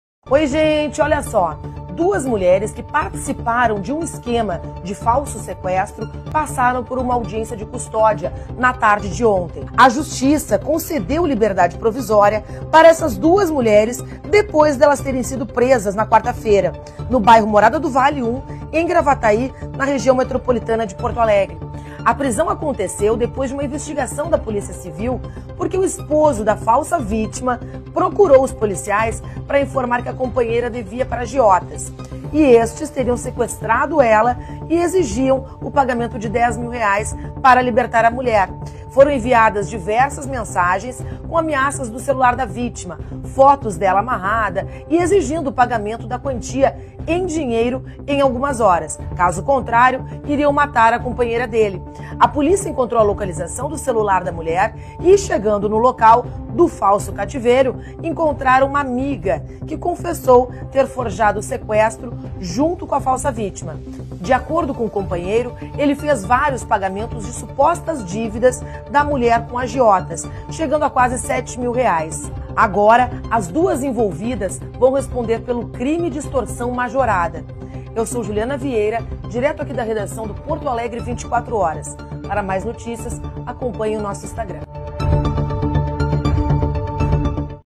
Duas mulheres são presas por esquema de falso sequestro!
Duas mulheres que participaram de um esquema de falso sequestro passaram por uma audiência de custódia na tarde de quinta-feira. A justiça concedeu liberdade provisória para as duas mulheres depois delas terem sido presas na quarta-feira (6), no bairro Morada do Vale I, em Gravataí, na Região Metropolitana de Porto alegre.